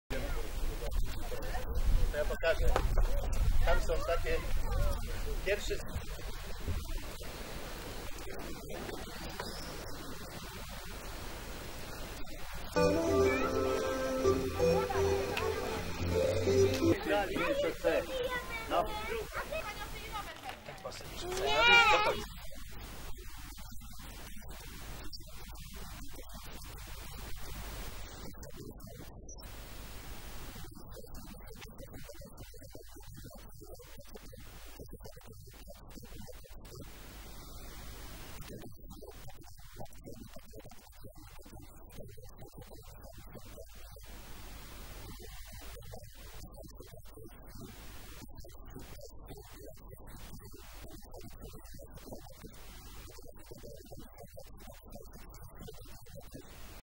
Ożarów Mazowiecki rolkami stoi
Relacja TV Misericordia z zawodów w jeździe na rolkach. Dobre media z dobrym przesłaniem. Telewizja Misericordia.